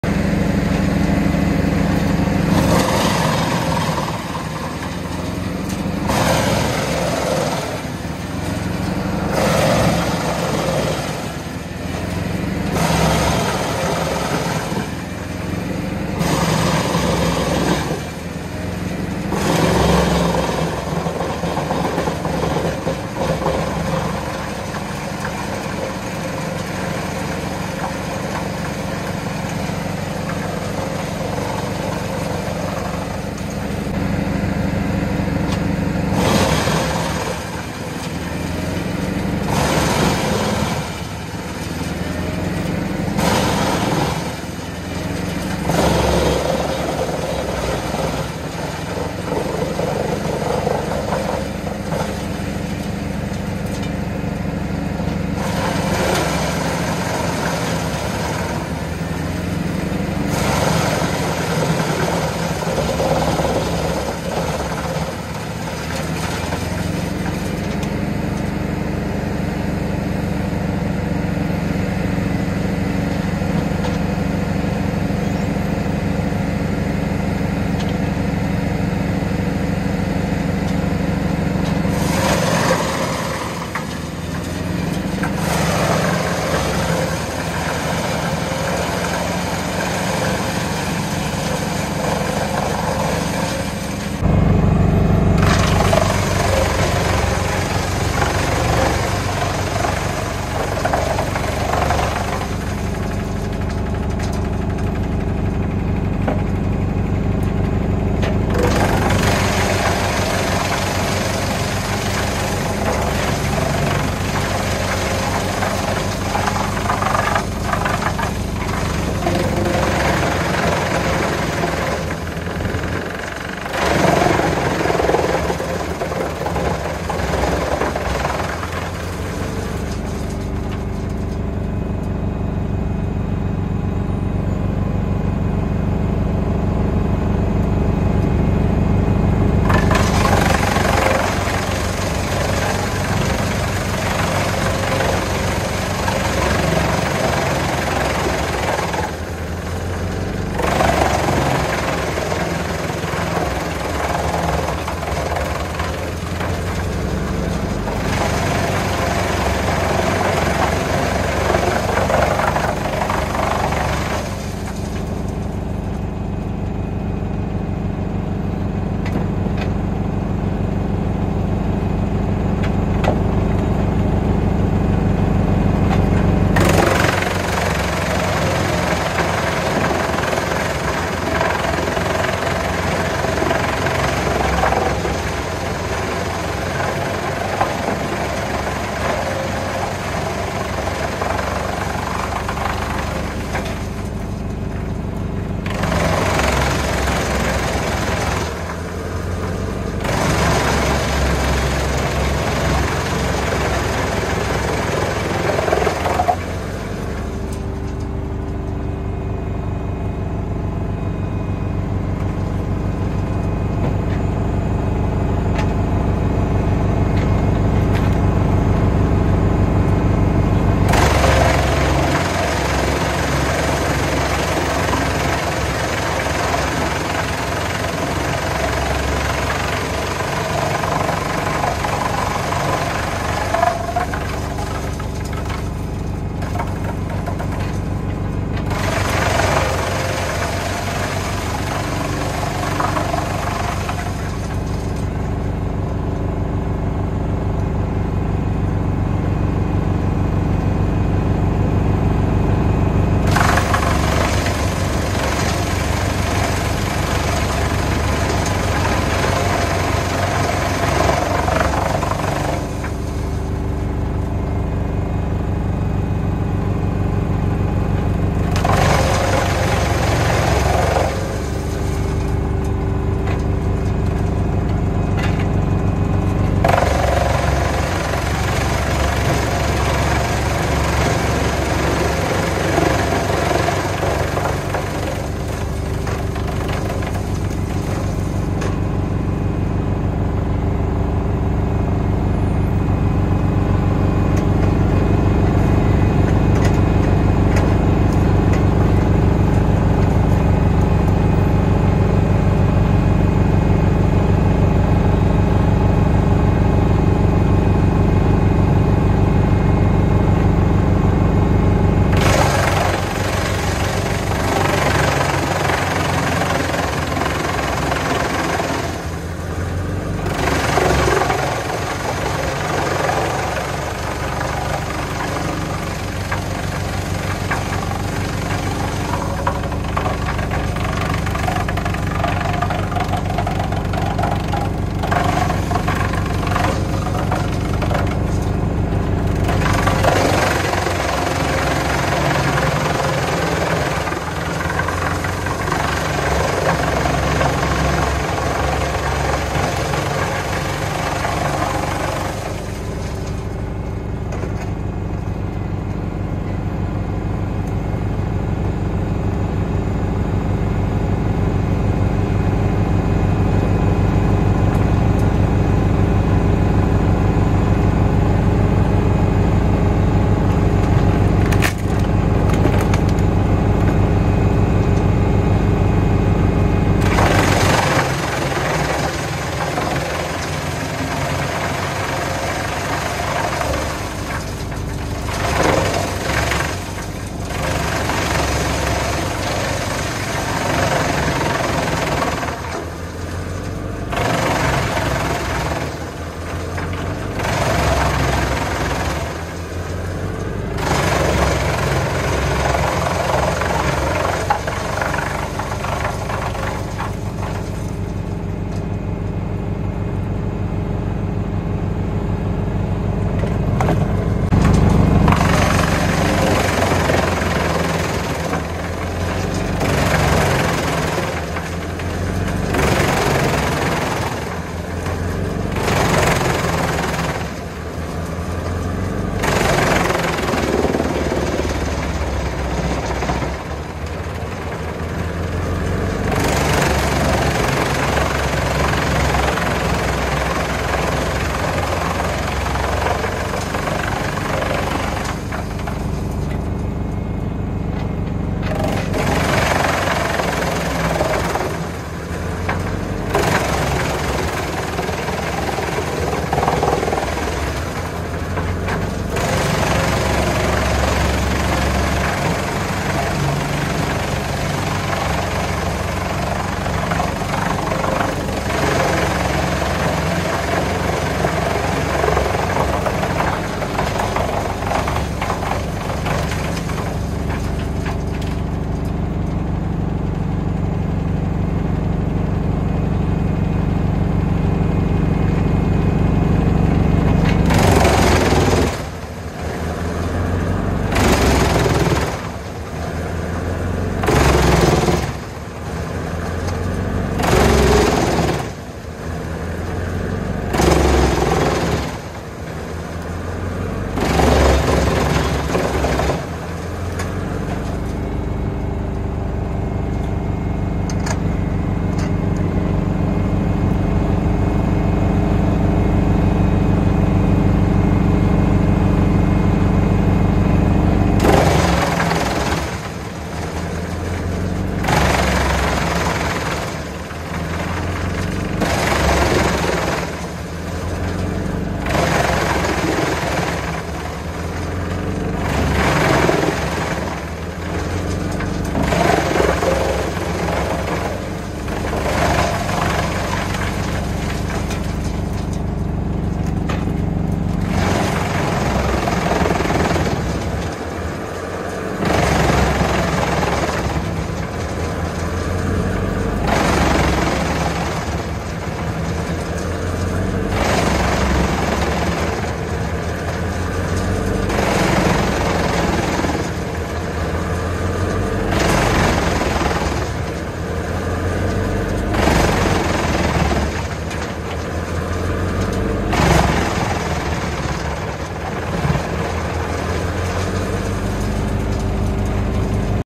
Haketusta Junkkari HJ500 hakkurilla ja Valtra T190 Traktorilla.
Videolla näkyvät koneet:

Valtra T190
Junkkari HJ 500
Kesla 305T